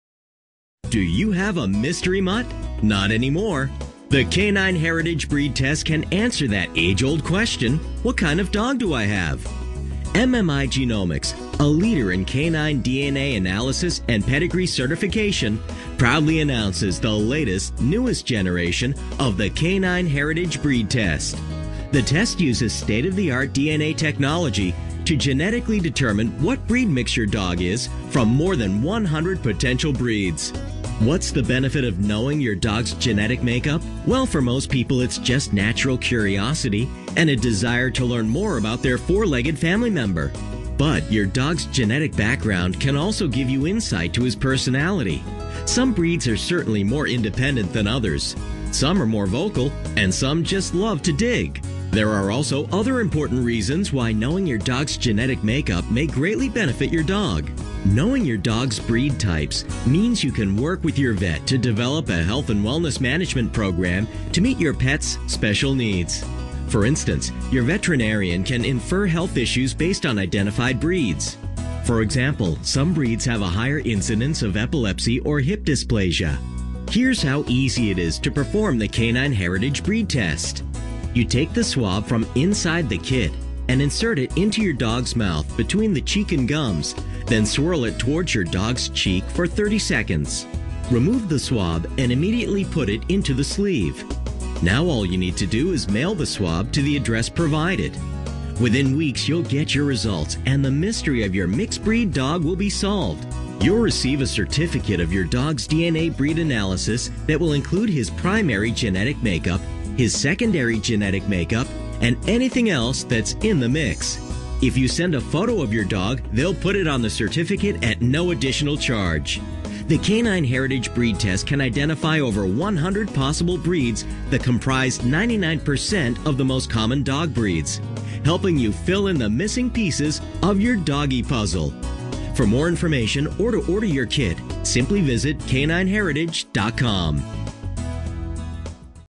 How to Determine Species of a Mutt via Dog DNA Testing
You can unveil the identify of your mixed breed dog via Canine Heritage Breed Test XL. For more information on how to determine species of a mutt,